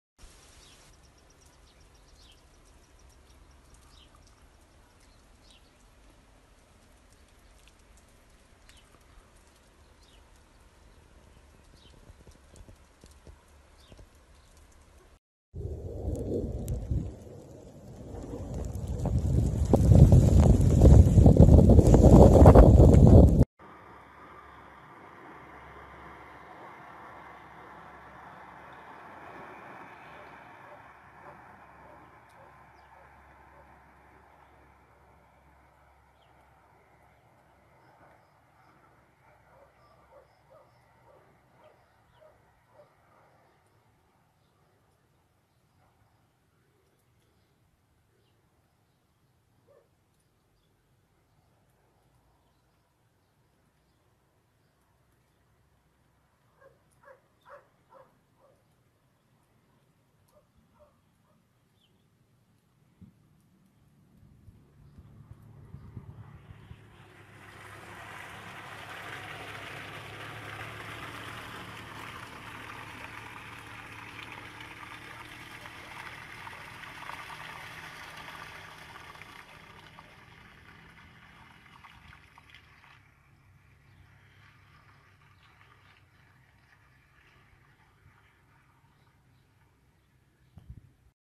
Nevada de Abril. Ibérica Riojana. 11/04/2018
Espectacular nevada en La Rioja. Imágenes de Yoana desde Villoslada, Agnieszka desde el Rasillo y Elisa desde Laguna